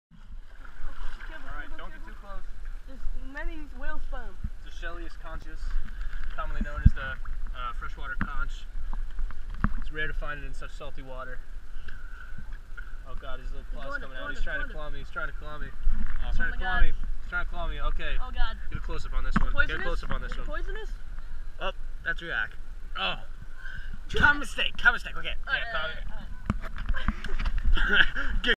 Finding a Wild Conch [GONE WRONG]
Three lads make an error.

Part of a nature documentary.
Grand Cayman Island, spring break of 2015 or 2016?